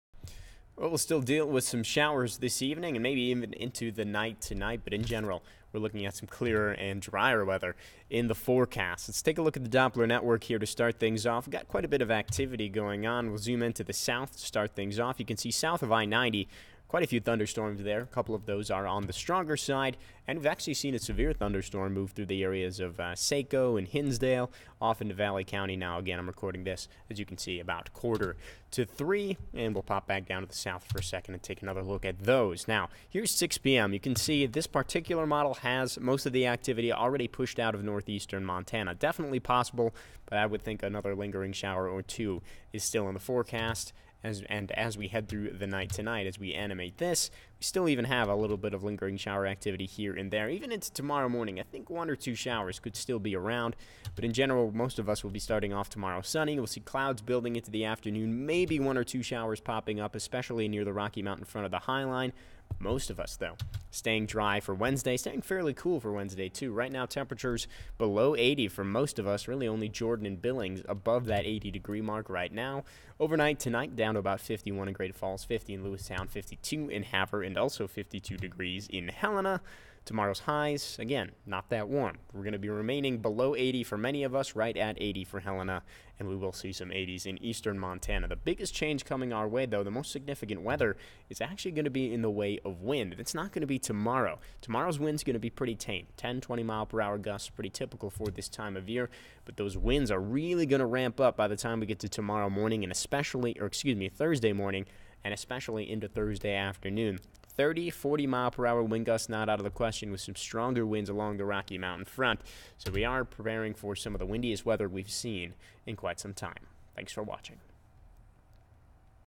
Calm weather Wednesday
Rain chances die down for the rest of the week